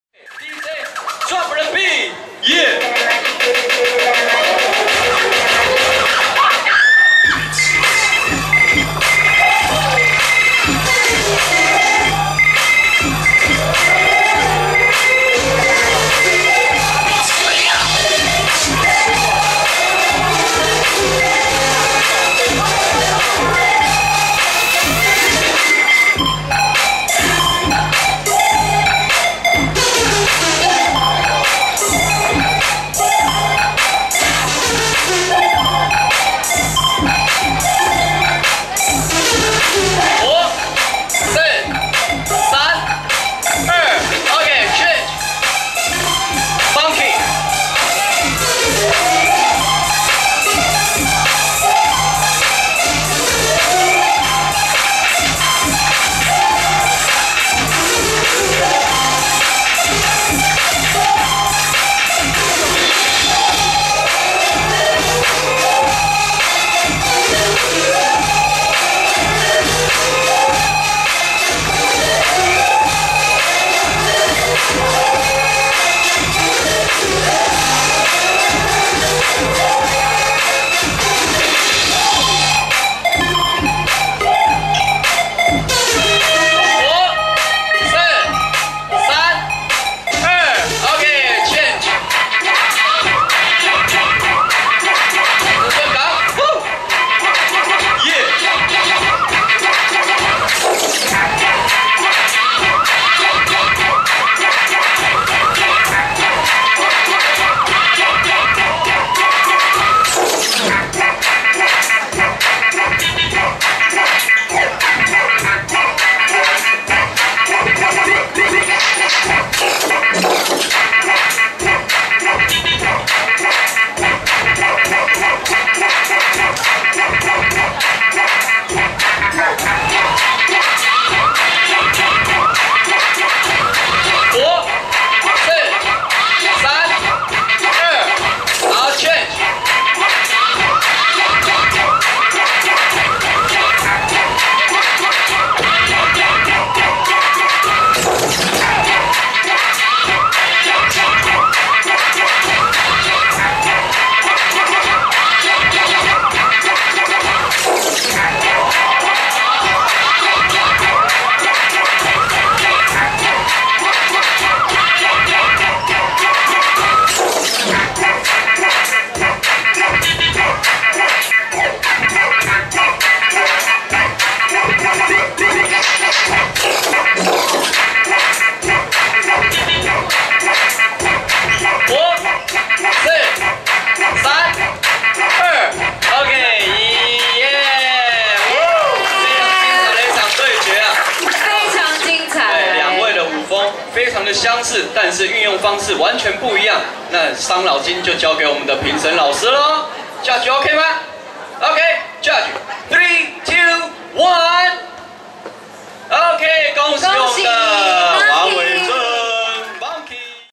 20140927 租稅盃街舞battle-決戰中台灣 個人賽八取四強戰~曾順港 vs Monkey
鏡頭左邊：曾順港
鏡頭右邊：Monkey(晉級)